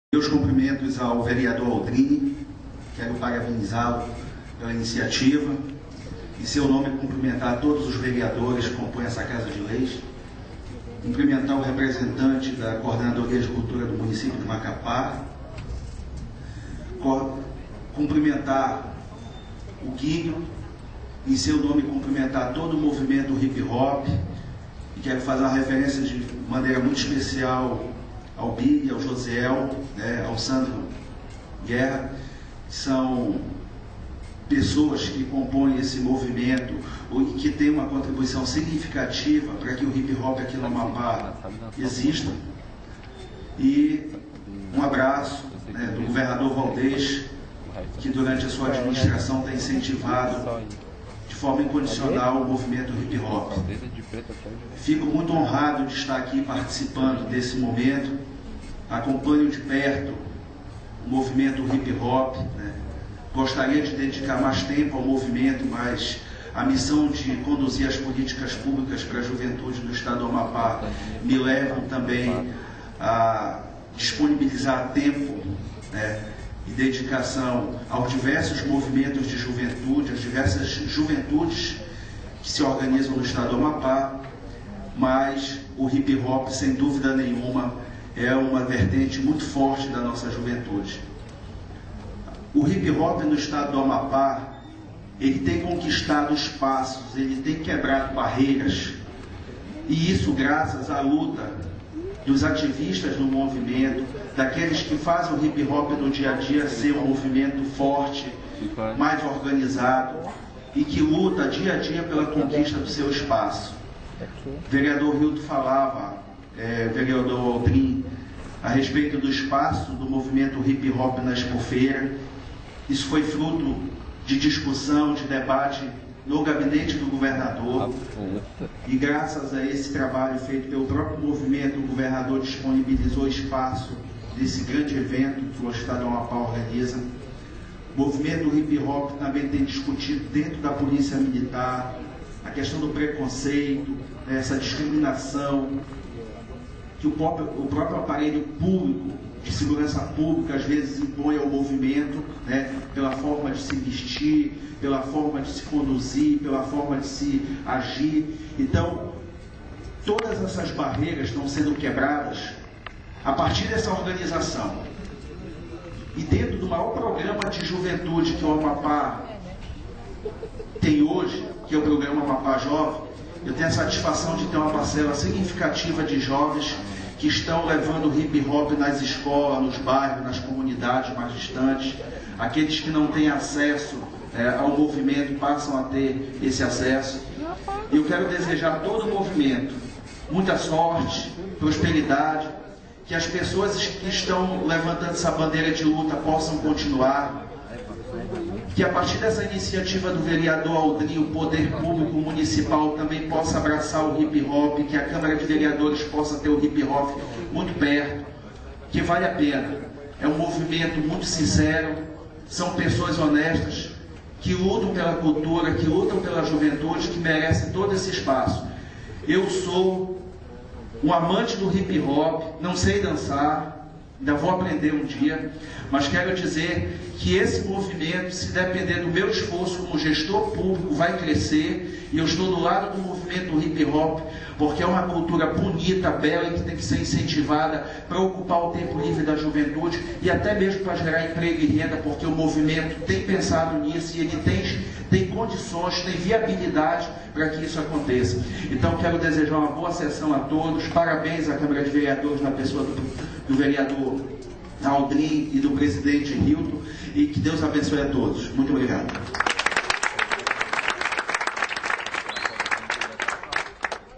dia mundial do hip hop parte I.wmv
DIA MUNDIAL DO HIP HOP NA CÂMARA MUNICIPAL DE MACAPÁ, NO DIA 12/11/2009, COM OBJETIVO DE HOMENAGEAR OS PIONEIROS DA CULTURA HIP HOP DO ESTADO DO AMAPÁ. FORAM ELES, PERNALONGA, JOÃO, BILHE, JOJO, GUINHO BBOY, HELDER, ANIRALDO, DICO. MAIS UMA BARREIRA QUEBRADA NA NOSSA SOCIEDADE. ESSA INICIATIVA FOI DA FADRO B.BOYS EM PARCERIA COM A CIA MACAPÁ BREAK